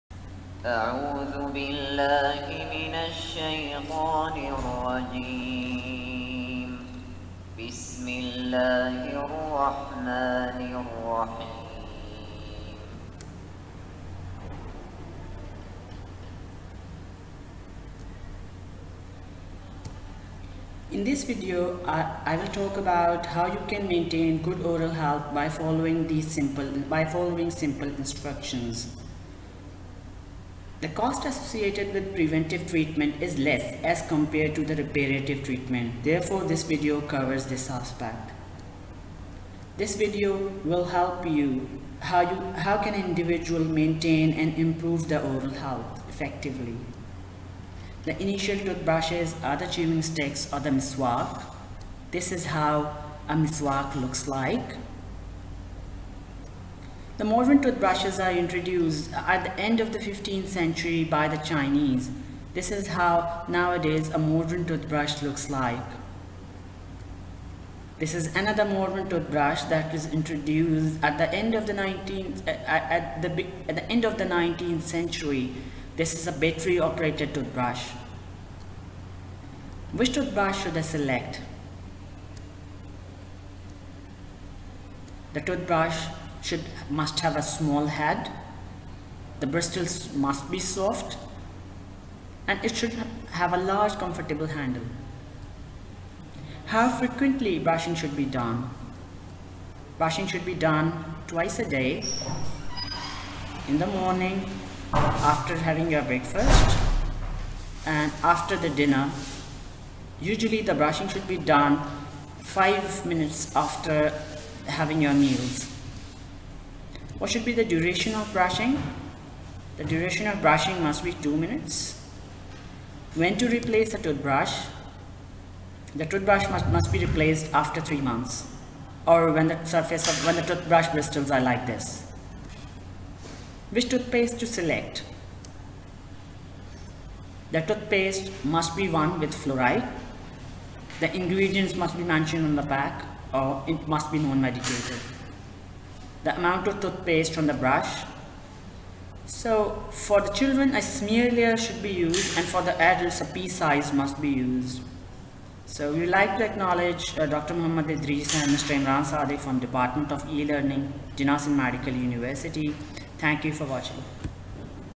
Maintainance of Oral Health Basic Concepts Dr Rizwan
This video will help the viewers in maintenance of good oral health.
CONTENTS OF THIS VIDEO LECTURE
00:00 - Introduction
0:47 - Initial Toothbrushes
0:57 – Modern Toothbrushes
1:19 – Which tooth brush an individual should select?
1:36 – Frequency of Tooth brushing
1:56 – Duration of Tooth brushing
2:02 -  When to replace your toothbrush?
2:13 – Toothpaste selection
2:25 -  Amount of toothpaste to be used